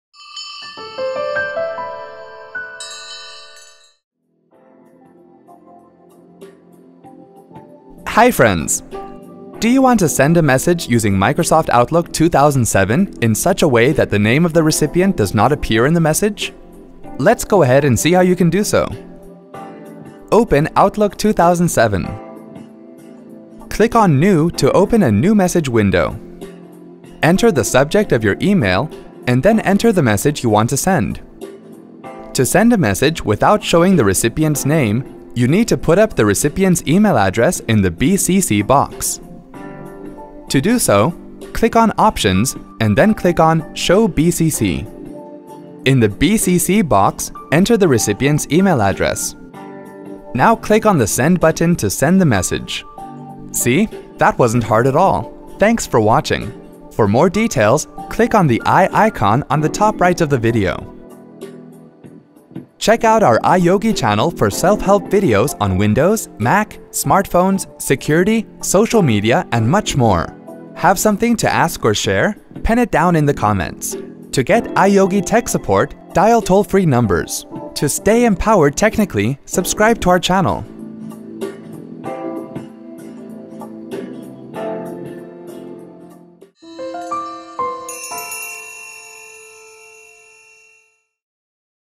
How to send a message without the recipient name showing in Microsoft® Outlook 2007 :Tutorial
Do you want to send a message using Microsoft® Outlook 2007 in such a way that the name of recipient does not appear in the message? If you want to send a message without the recipient name showing in Microsoft® Outlook 2007, watch this video and follow the steps to do so.